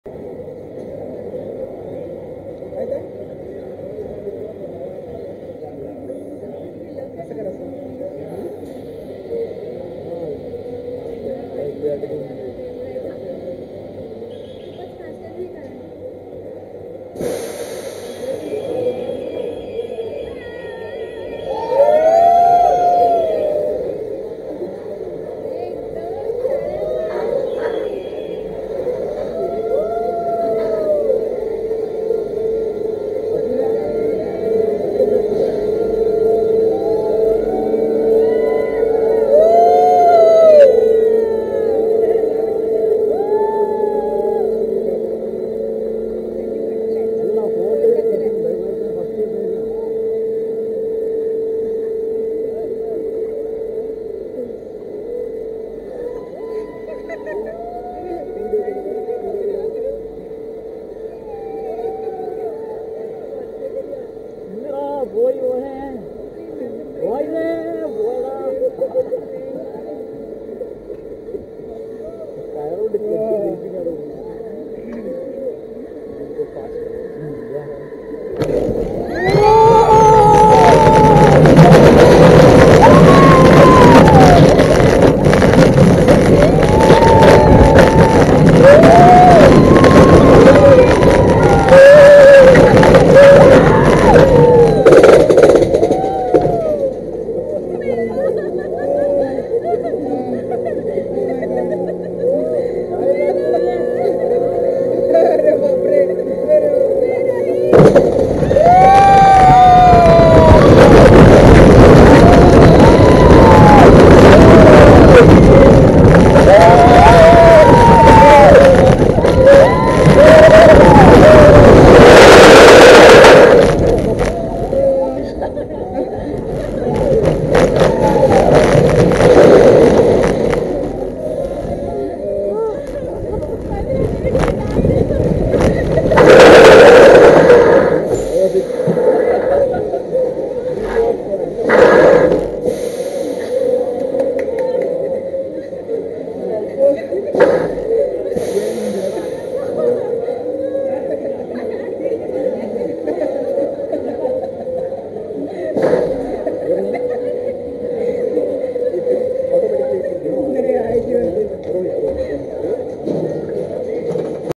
Sunkid RECOIL Roller Coster | Wonderla Amusement Park | Wonderla Bangalore
Wonderla Amusement Park

Wonderla Amusement Park is India's Famous Amusement Park having many thrilling rides. Recoil experience was the most thrilling one. Enjoyyyy!!!

Wonderla Amusement Park